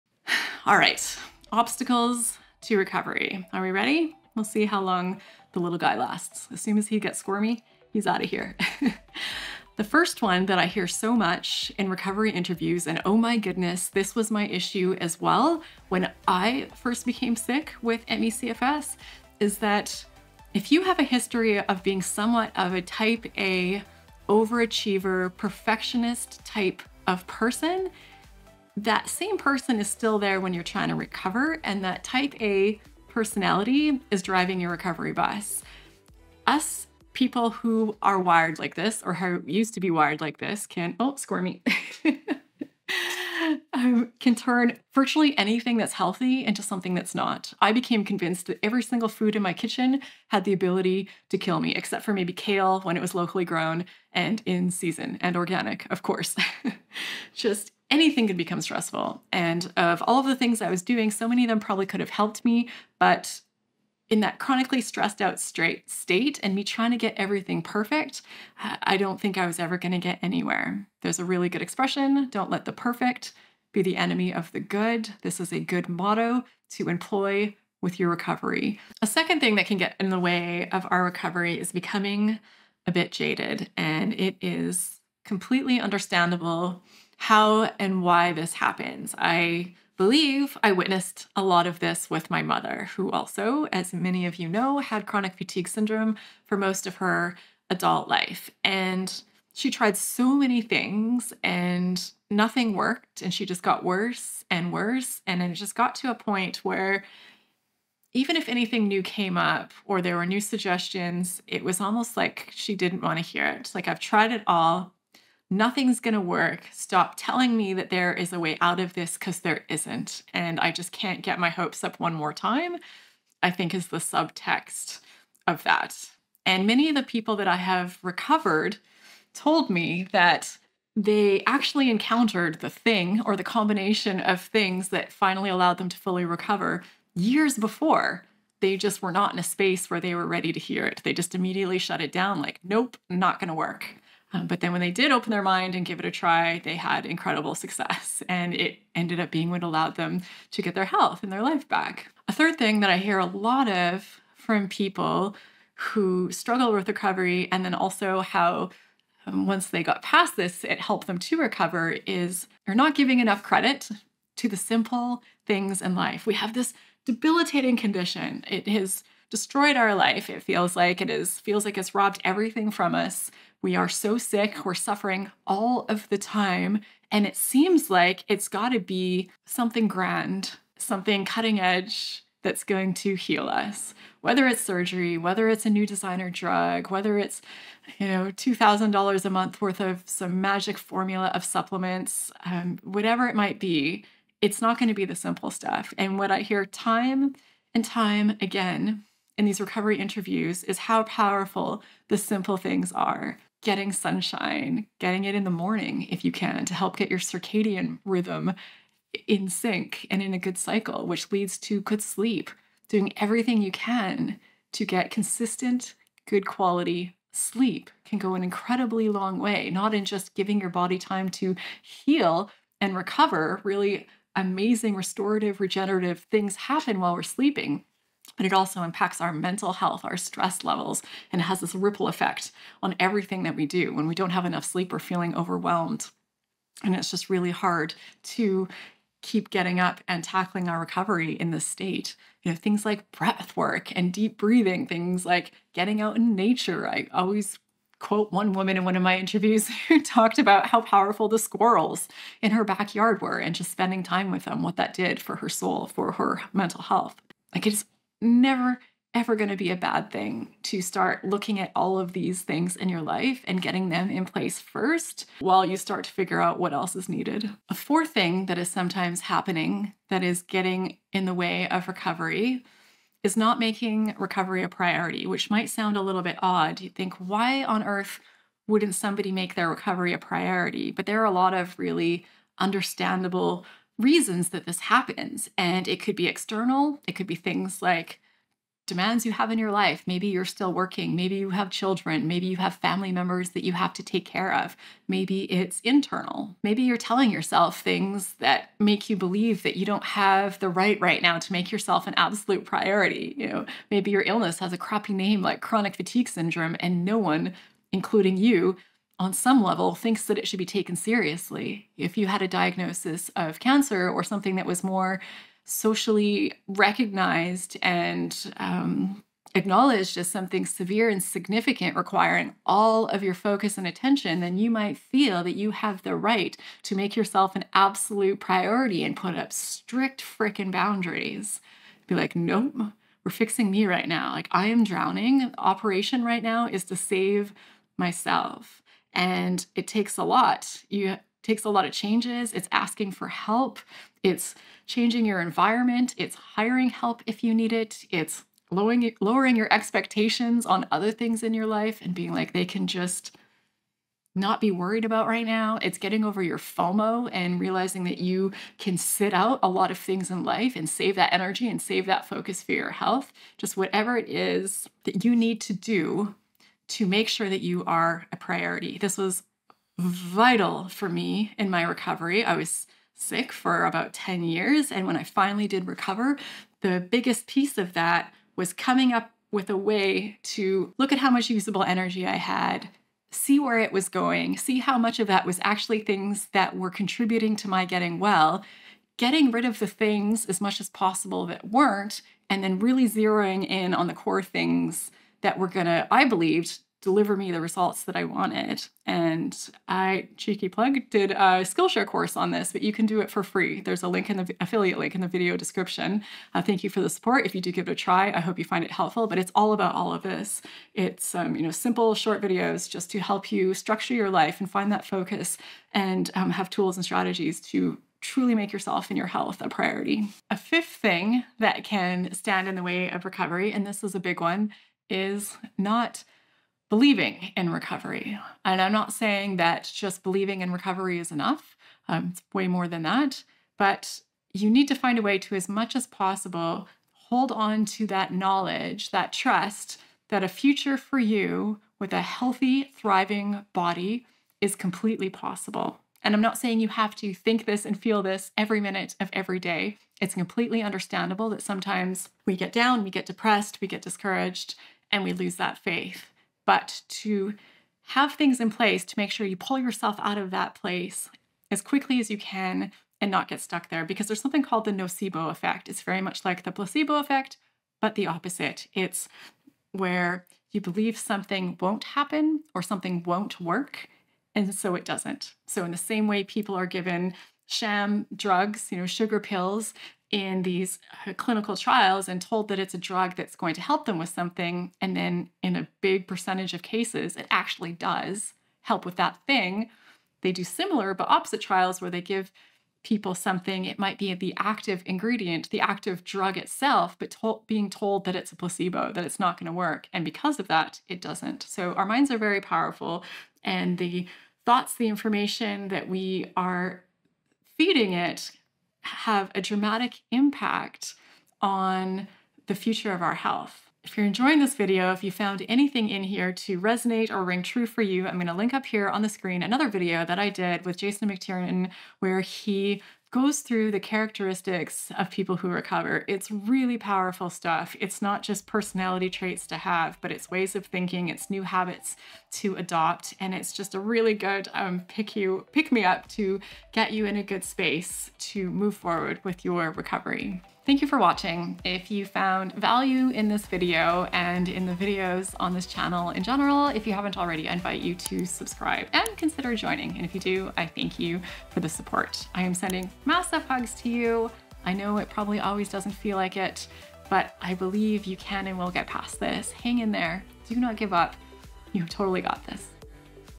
5 Pitfalls & 3 Must-Dos for CFS & Long Covid Recovery
In this video, I get real about the roadblocks I encountered during my recovery journey from ME/CFS over 10 years, and the common setbacks I've heard time and again from over a hundred people I've interviewed on my channel.

MENTIONED IN THIS VIDEO:
- The Vital TOP 5 Takeaways

Finding Freedom: Escaping From the Prison of Chronic Fatigue Syndrome. View on Amazon

WANT TO BE A GUEST ON MY YOUTUBE CHANNEL?

TIME STAMPS:
00:00 - 🚀 The Top 5 Recovery Obstacles You Must Know
00:10 - 🚌 Who's REALLY Driving Your Recovery Bus?
01:19 - 😒 The Danger Zone: Why Getting Jaded May Shut You Down
02:30 - 🔍 Missed Magic: Are Simple Tools Slipping Under Your Radar?
04:07 - 🐿️ Rachel's Squirrels: A Game-Changer Story
04:27 - 💡 Self-Check: Is Bouncing Back to Health Your #1 Goal?
05:03 - 🤔 How a Crappy Illness Name Might Be Tripping You Up
07:00 - 💭 Do You Need a Mindset Makeover? Let's Dive In!
07:42 - 💊 Placebo vs. Nocebo: Which Side Are You On?